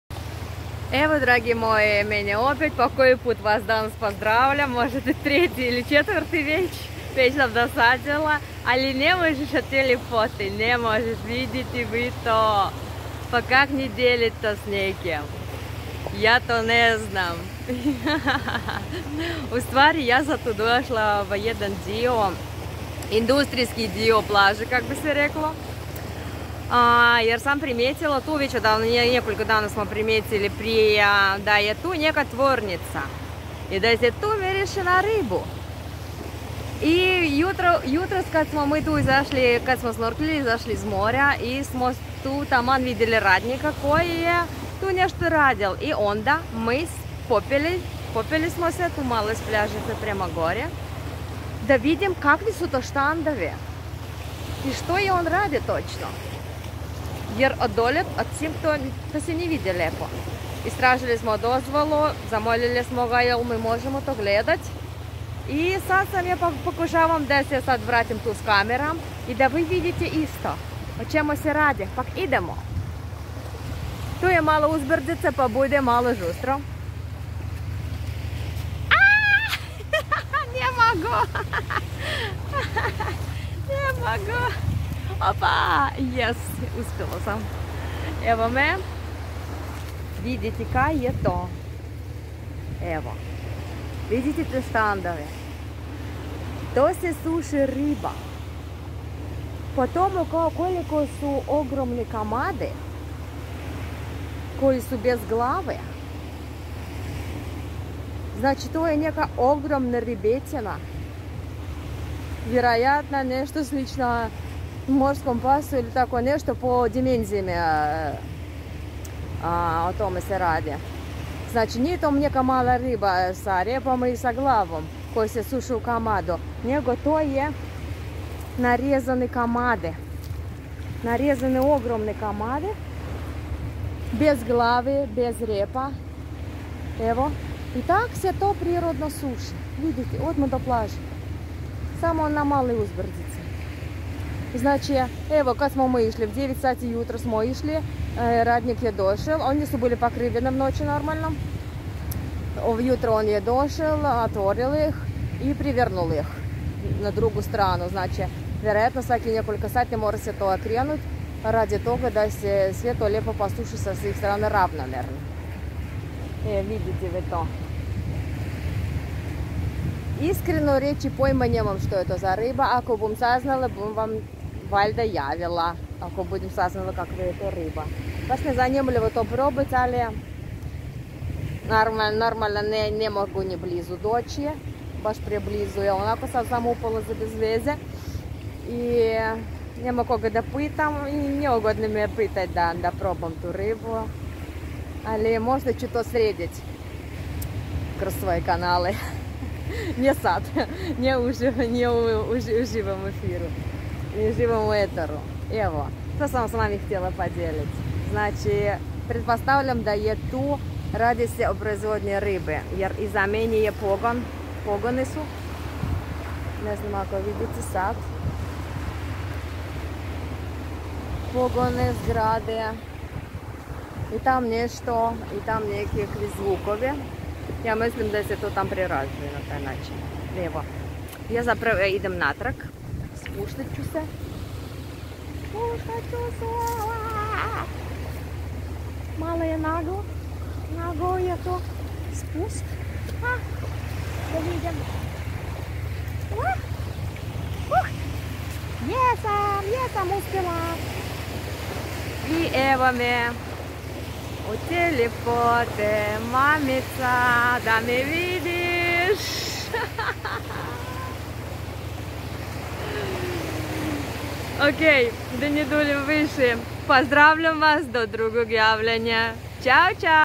MALDIVE🏝 SUSIONICA RIBEEEE👈🐠🐠🐠🐋🐋🐋
SLUCAJNO SAM NAISLA NA PRIRODNU SUSIONICU RIBE,KOJA SE ZOVE SELLFISH🐋 I,KAKO SU MI REKLI,DOSEZE DO 4 METARA DUZINE🤯🤯🤯🐋🐋🐋